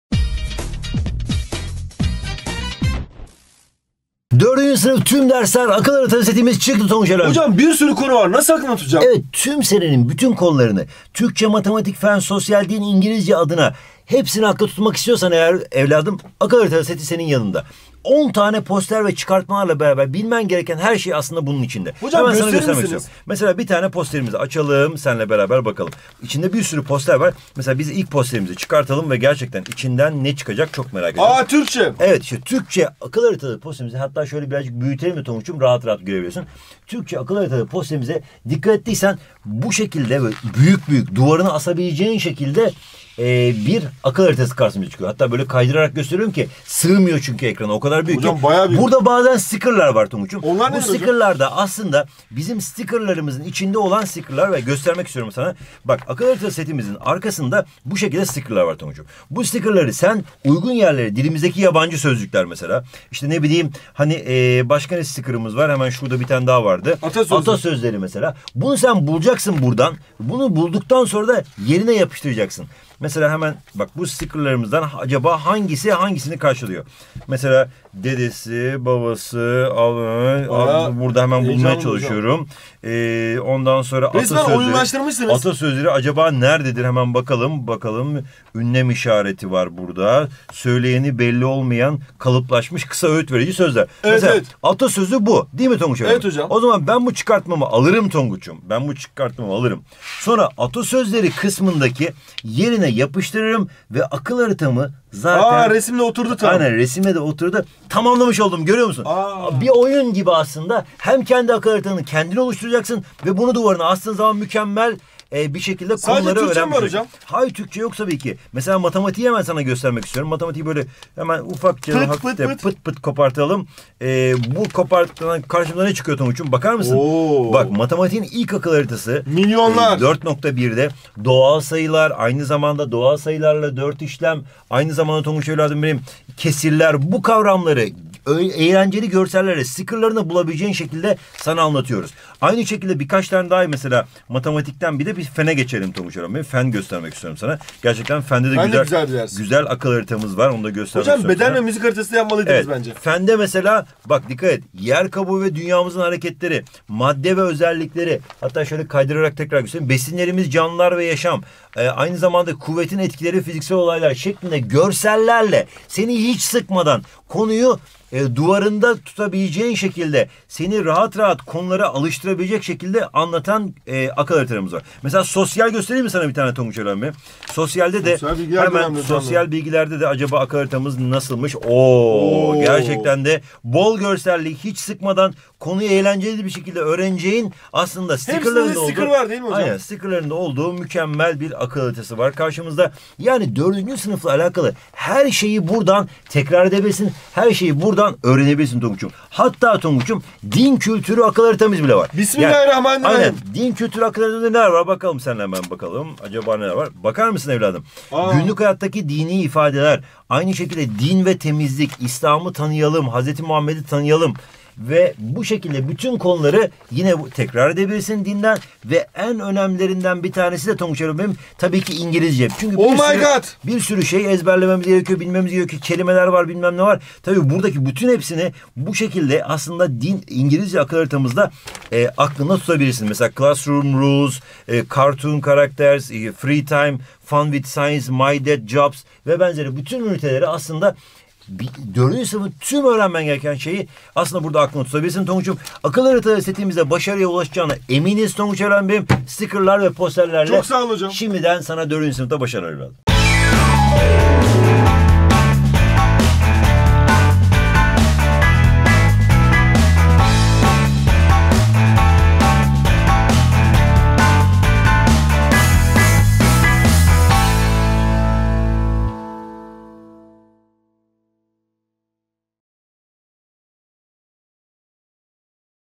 4. Sınıf Akıl Haritaları Seti ÇIKTI!
4. Sınıf Akıl Haritaları Seti ÇIKTI!Detaylı incelemek ve sahip olmak için hemen buraya tıklayabilirsin
4. Sınıf Akıl Haritaları ile okul derslerinin tüm konularını tek bir posterde görebileceksin. Haritalar, bu sene okulda göreceğin derslerle ilgili görseller, kısaltmalar ve konuların temelini içeren özet bilgi notlarını içeriyor. İçerdiği temel ve öz bilgileri bol görselli anlatımlarla destekliyor. 😎